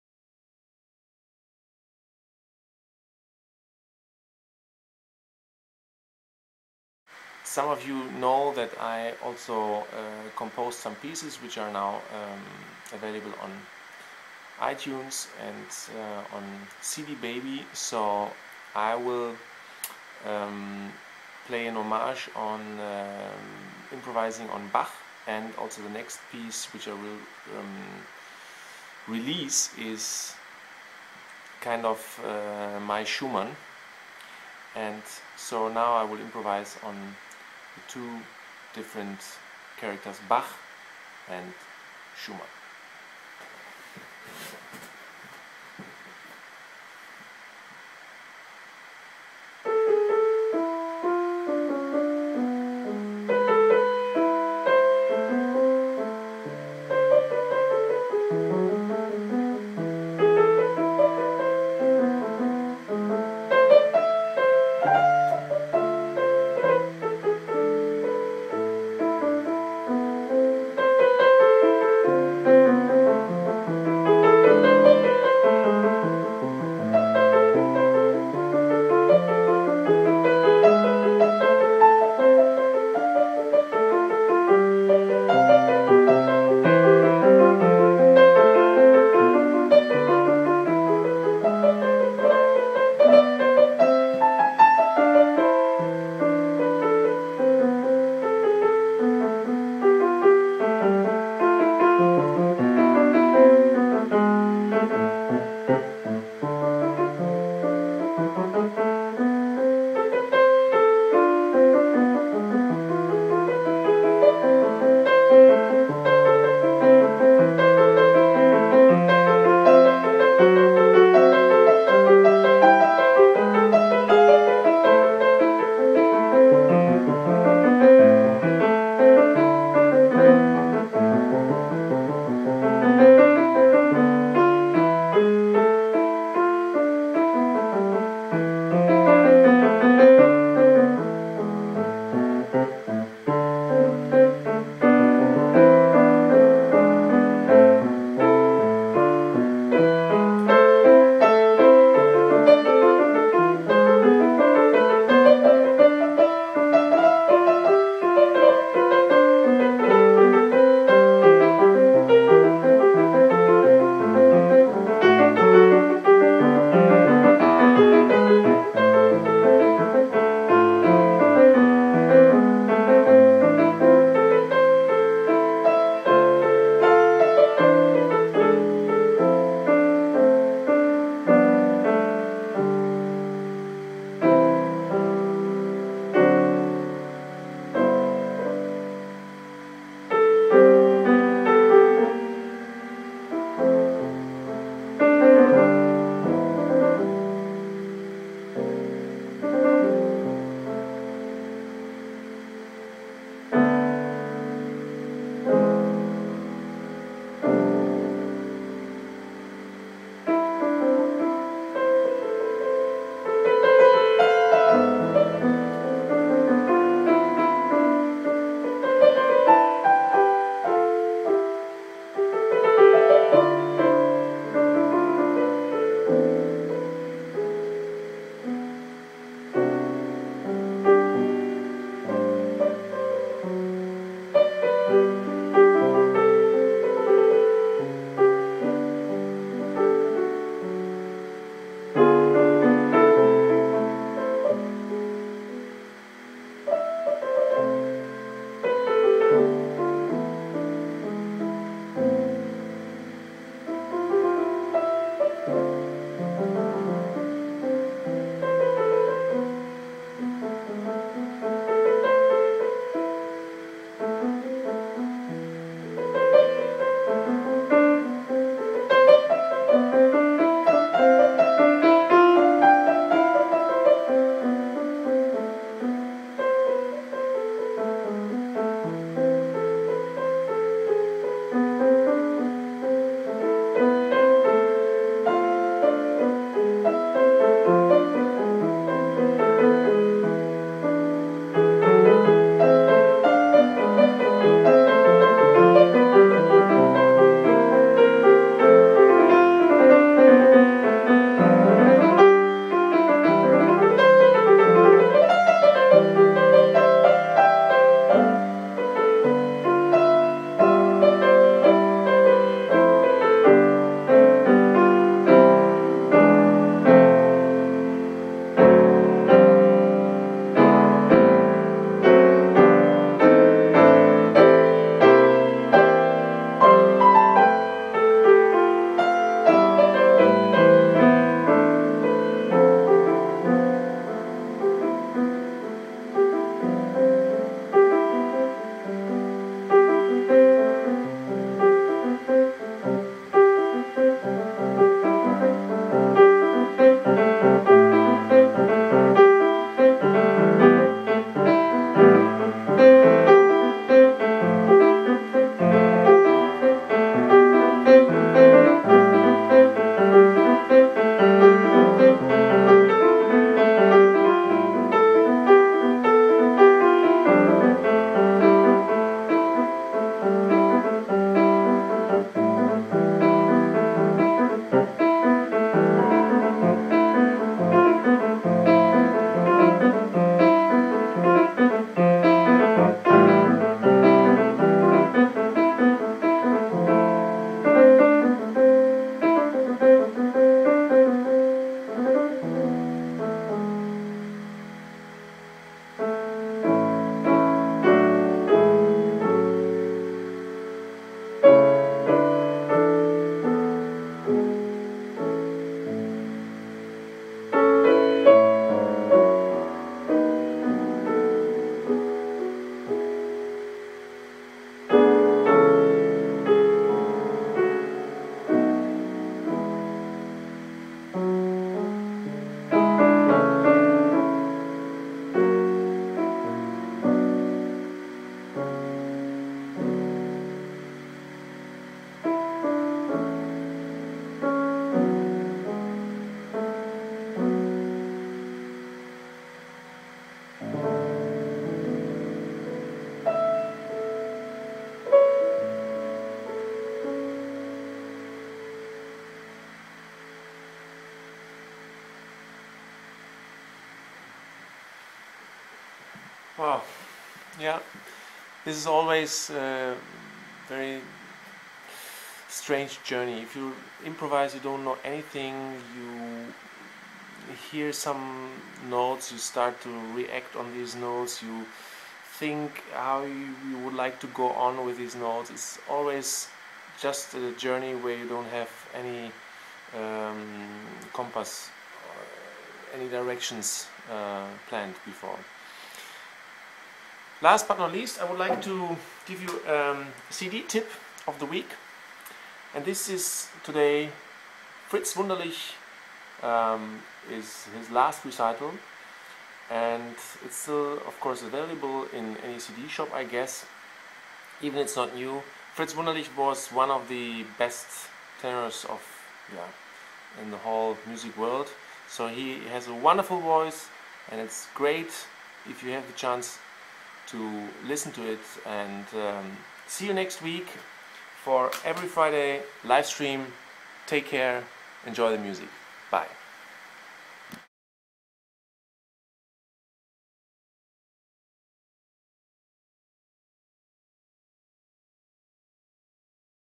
Andreas Kern: BACH AND SCHUMANN
An improvisation on Johann Sebastian Bach and Robert Schumann.